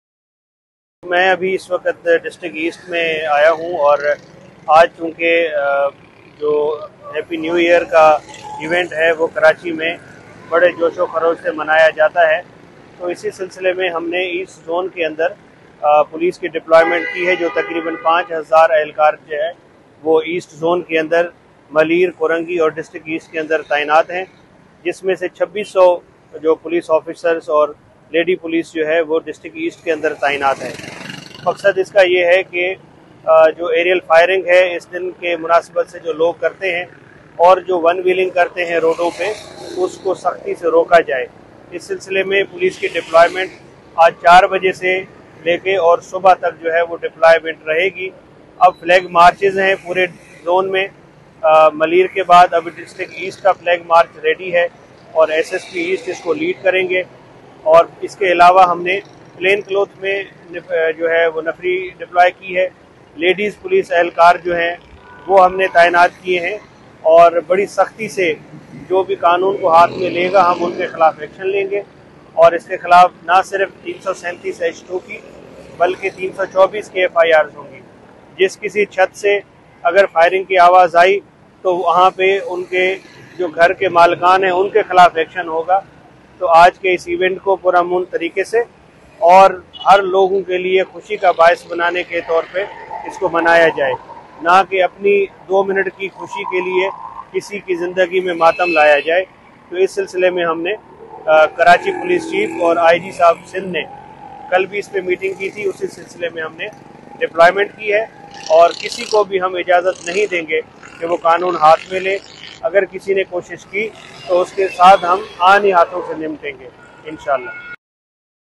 DIG EAST ZONE AZFAR MAHESAR
کراچی: نیو ائیر نائٹ کی مناسبت سے ڈی آئی جی ایسٹ غلام اظفر مہیسر کا مختلف علاقوں کا دورہ

ڈی آئی جی ایسٹ ڈسٹرکٹ کورنگی، ڈسٹرکٹ ملیر اور ڈسٹرکٹ ایسٹ میں خود جاکر سیکیورٹی انتظامات کا جائزہ لیا، ترجمان

تینوں ضلعی ایس ایس پیز نے ڈی آئی جی ایسٹ کو علیحدہ علیحدہ سیکیورٹی انتظامات پر بریفنگ دی

ڈی آئی جی ایسٹ کا پولیس کی جانب سے نیو ائیر نائٹ پر کئے گئے سیکیورٹی انتظامات پر اظہارِ اطمینان

شہری نئے سال کا جشن قانون کے دائرے میں رہتے ہوئے منائیں، ڈی آئی جی ایسٹ کی شہریوں سے اپیل

نئے سال کے موقع پر دوسروں کے لئے تکلیف کا باعث نہ بنیں، ڈی آئی جی ایسٹ کا پیغام

شہری نئے سال کی آمد پر ہوائی فائرنگ سے باز رہیں، ہلڑ بازی اور پٹاخے پھوڑ کر دوسروں کے لئے اذیت کا باعث نہ بنیں، ڈی آئی جی ایسٹ کا پیغام